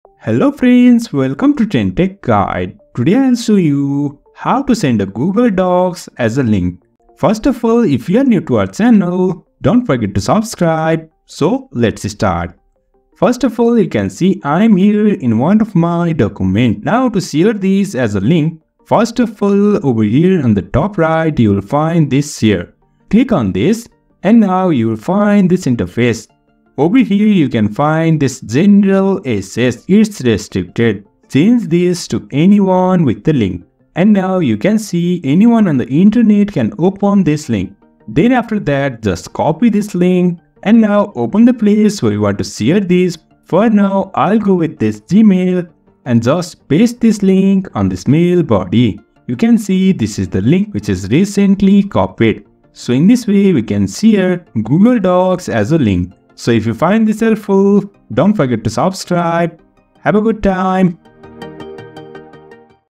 How to Send Google Docs as a Link | Share Documents Easily
Learn how to quickly share your Google Docs as a link with others. This tutorial will show you the simple steps to generate a shareable link for your Google Docs, making it easy to collaborate and share documents.

👉 Explore more!